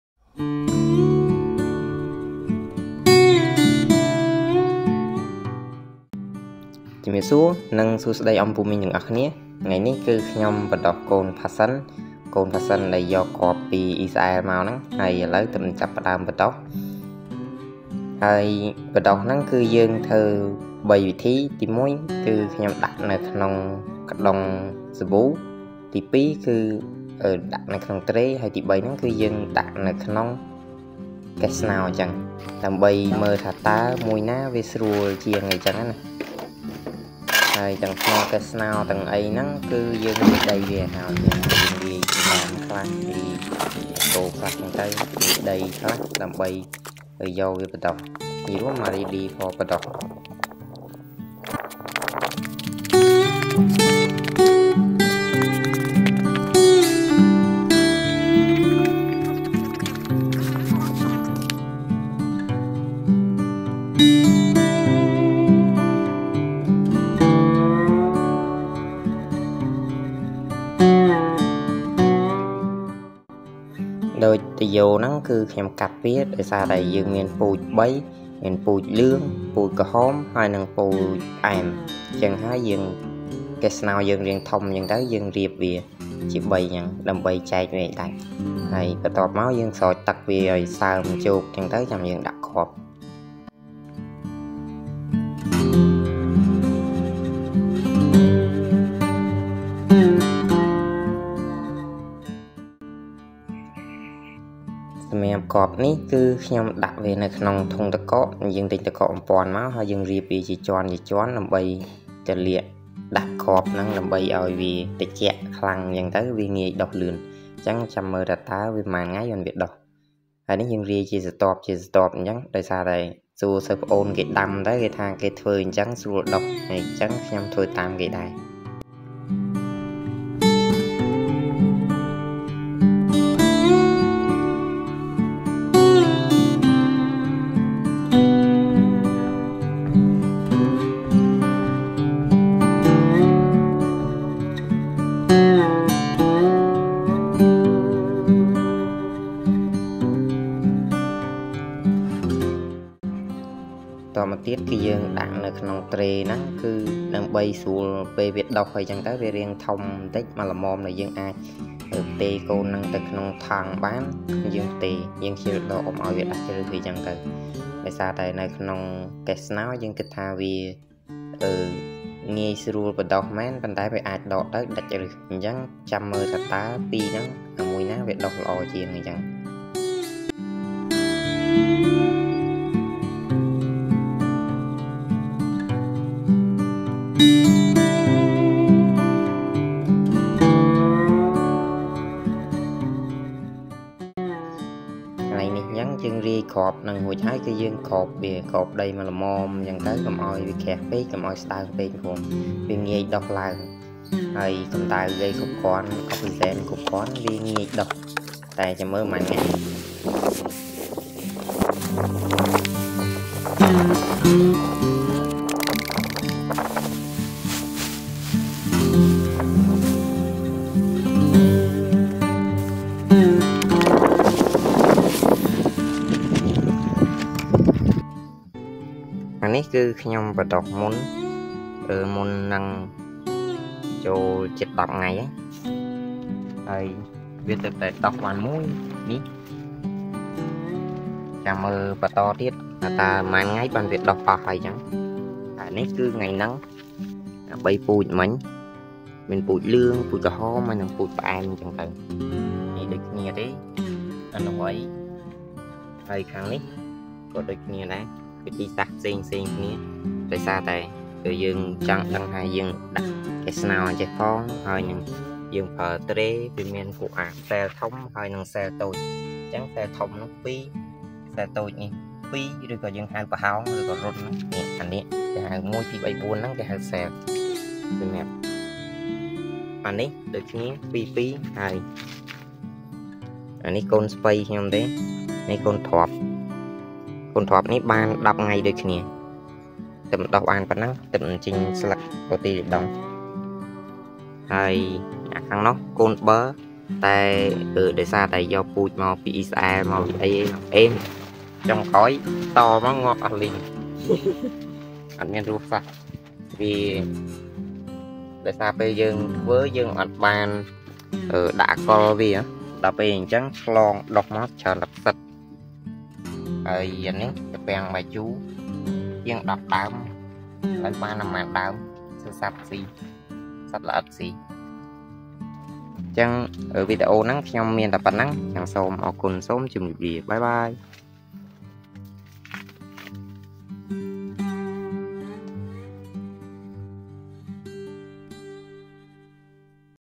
ការបណ្តុះគ្រាប់ផាសិន/ How to grow passion fruit. 17 11 2023
នេះជាការបណ្តុះកូនផាសិនដែលខ្ញុំបានយកគ្រាប់ពីអ៉ីស្រាអែលមក ដែលមាន ពូជជូអែម និងពូជផ្អែម។ ហើយនេះជាការសាល្បងដាំលើដី២០អានៅភ្នំវល្លិ៍ខេត្តកែប